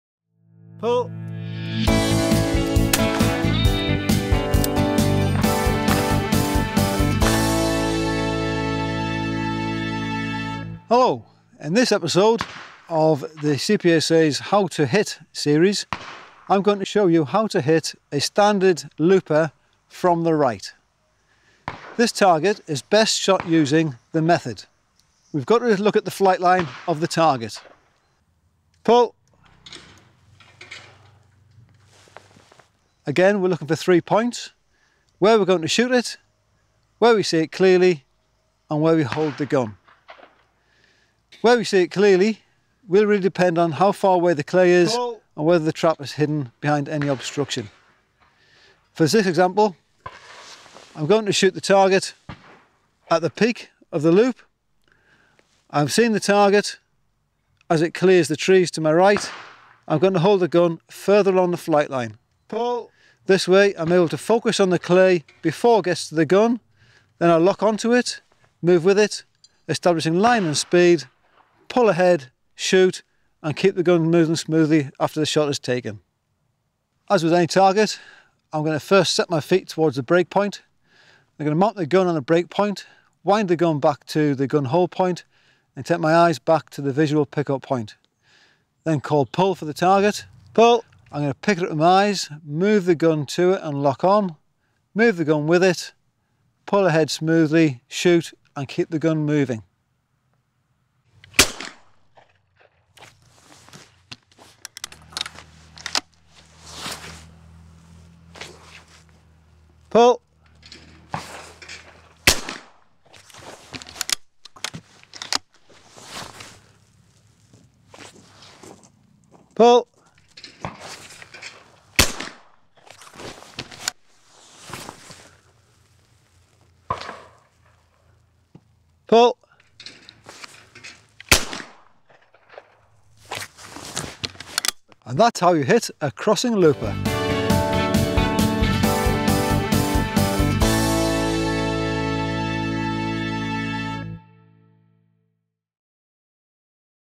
How to Hit Episode 4 'The Crossing Looper'
Episode 4 of the CPSA's series on tackling the most common Sporting Targets, presented by CPSA Level 3 Senior Coach Simon Arbuckle. This episode focuses on The Crossing Looper Target.